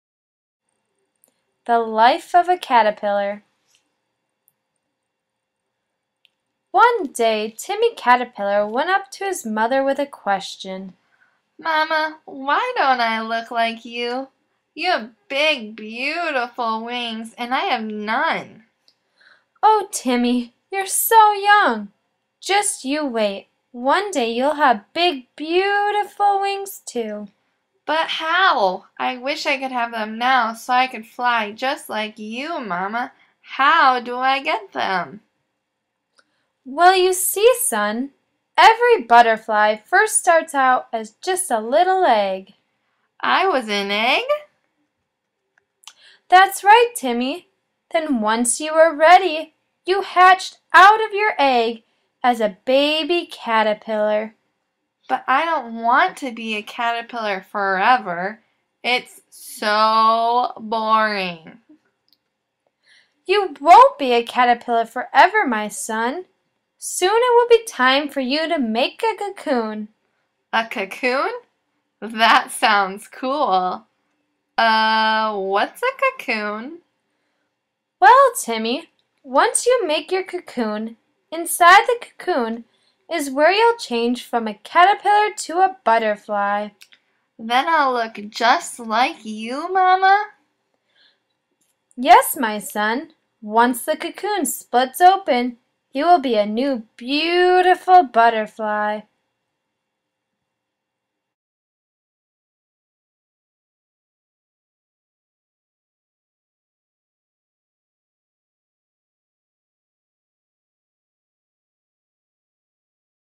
The Life of a Caterpillar
Little Timmy the caterpillar had a big question to ask his mother the butterfly. Timmy did not understand why he looked so much different than his own mother. He wanted to be able to fly just like her. Timmy's mother explains to him that one day he will have big beautiful wings too!